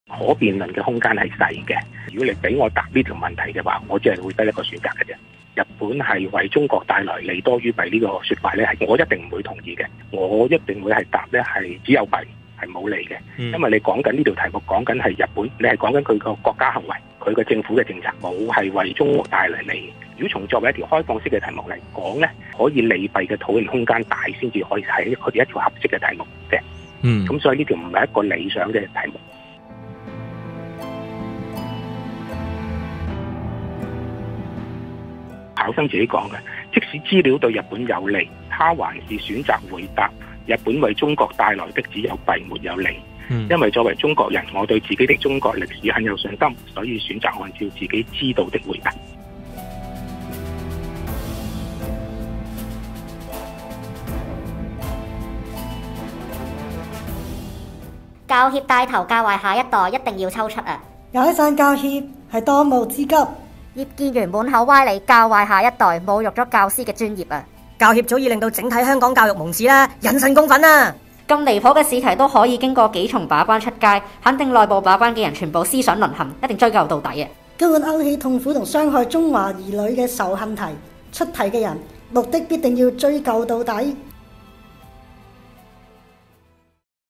【短片】【答案只得一個】中學校長會會長鄧振強指文憑試歷史科試題，要求學生回答「1900-45年間，日本為中國帶來的利多於弊？」非理想試題，因答案只得弊沒有利。不少網民要追究出題責任，狠批教協包庇出題者
//文憑試歷史科試題美化日本侵華史實，教育局要求考評局把整條題目取消。啲失德教師出埋哂呢啲題目真係害人不淺//